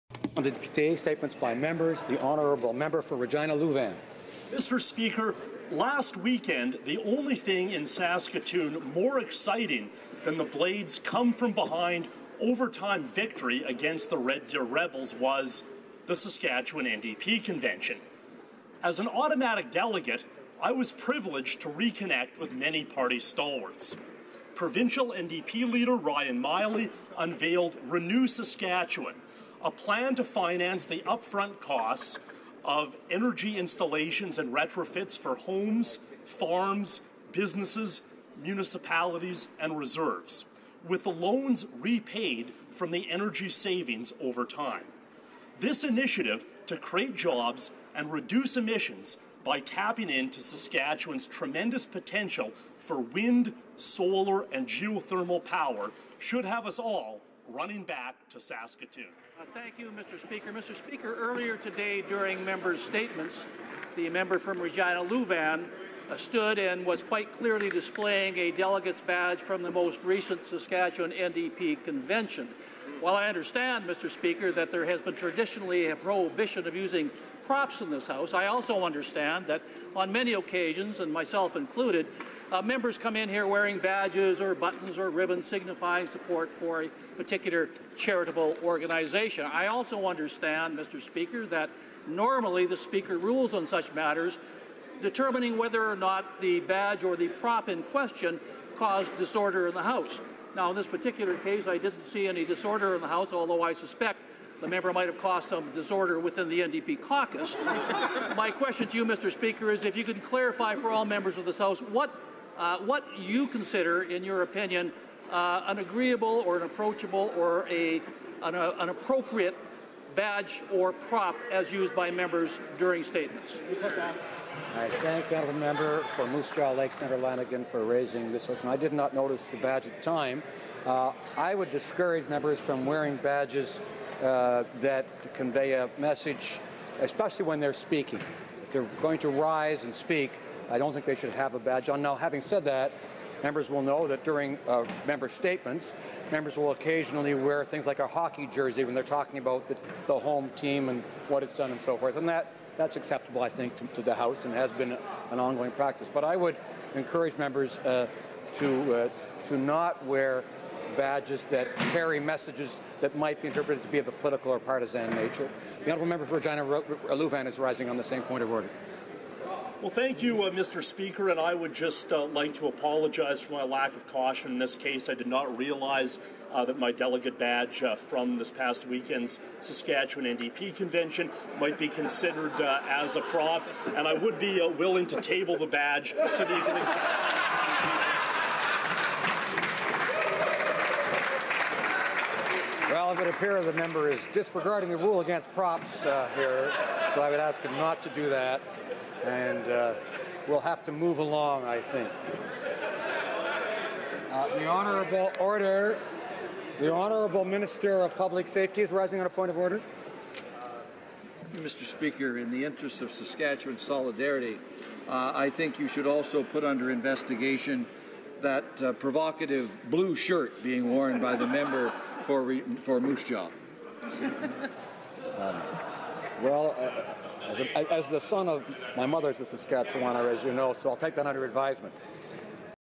MP Weir on Sask. NDP Convention (Oct. 16, 2018)
Regina–Lewvan MP Erin Weir is called to order for wearing his Saskatchewan NDP convention badge in the House of Commons.